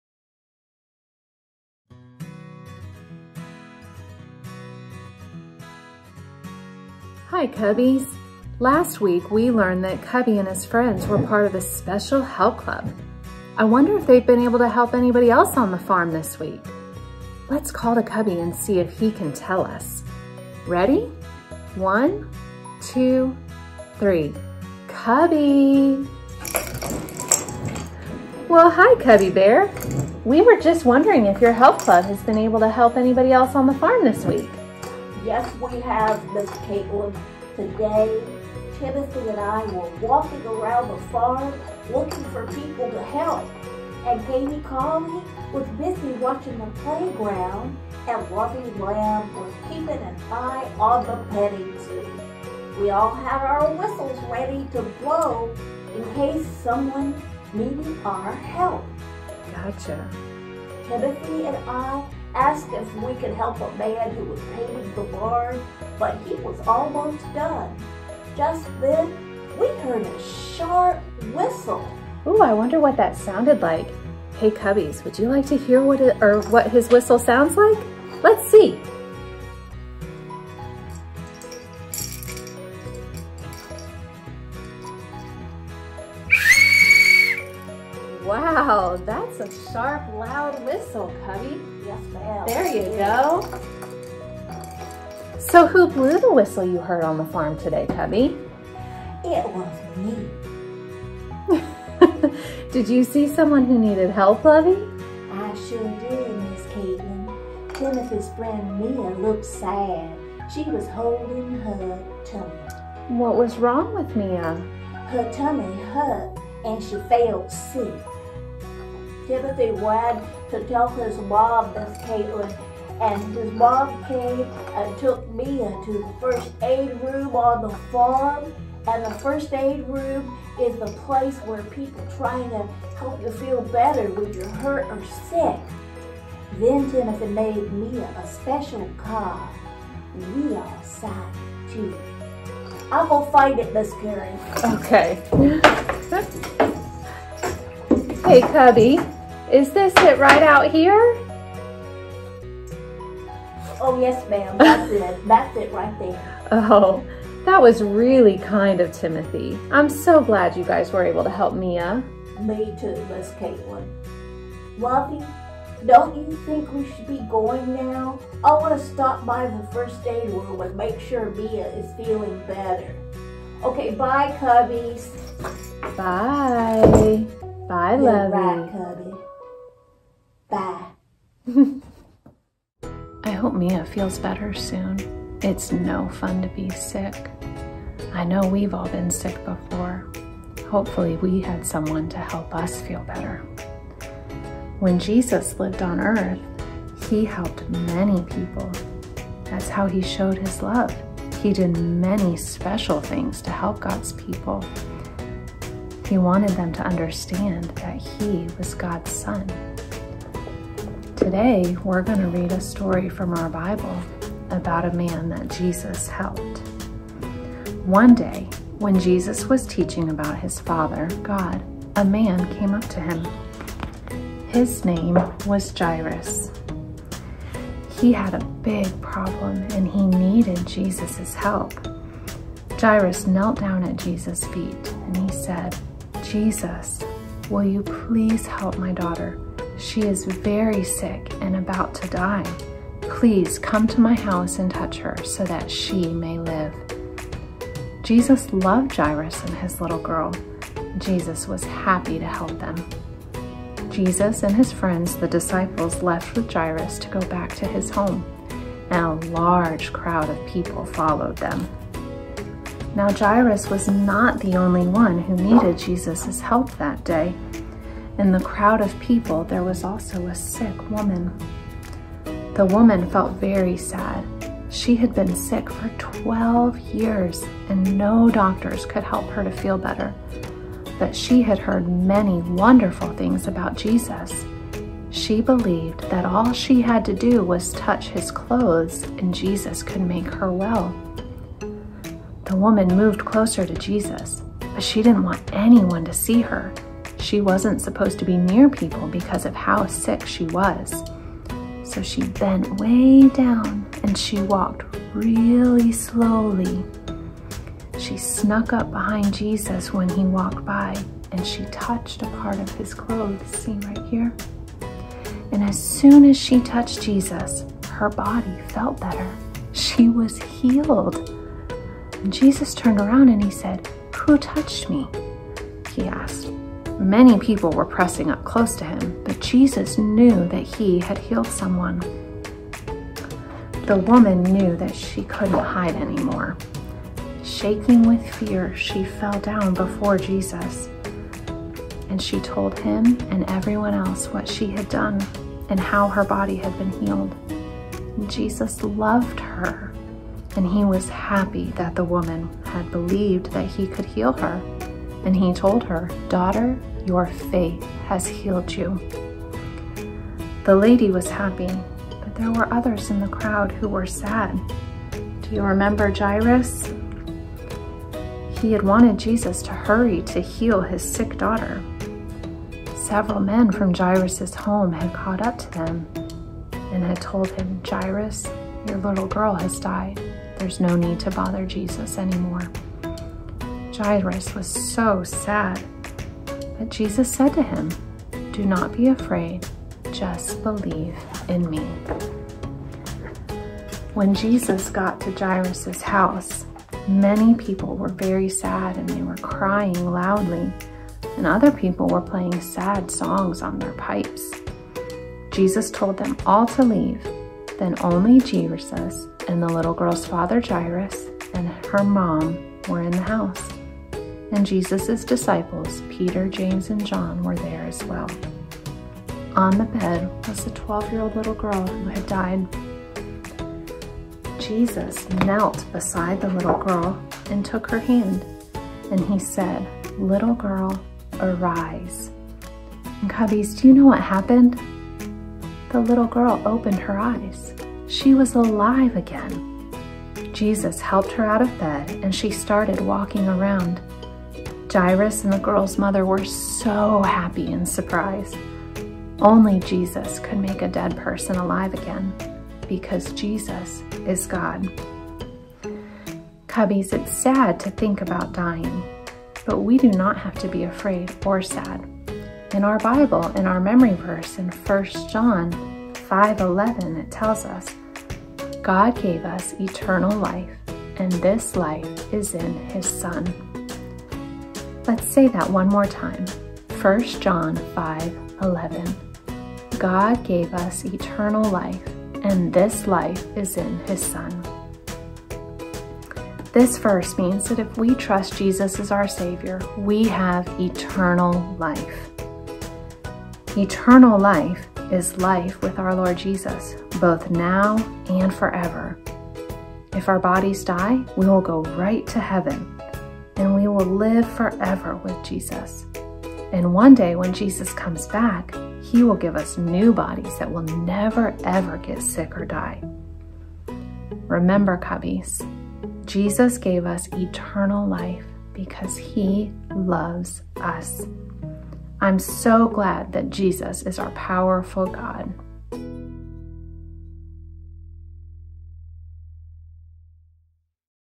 Cubbies 2.24.21
Large Group Lesson and Puppet Show
Awana Club of South Tampa Fellowship